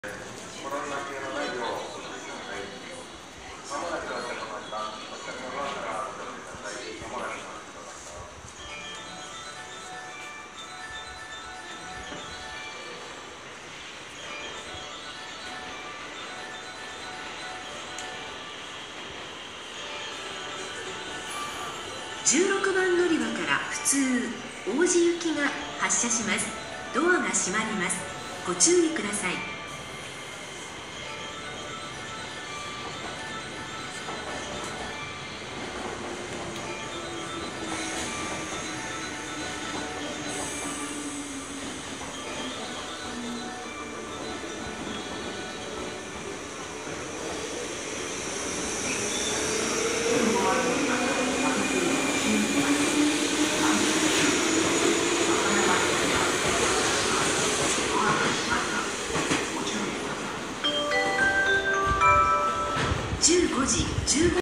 【4K】JR西日本201系 普通 王寺行き 発車シーン
201系(ND601編成)
普通 王寺行き(1798K)
2017年03月09日 西日本旅客鉄道関西本線 天王寺駅
発車シーン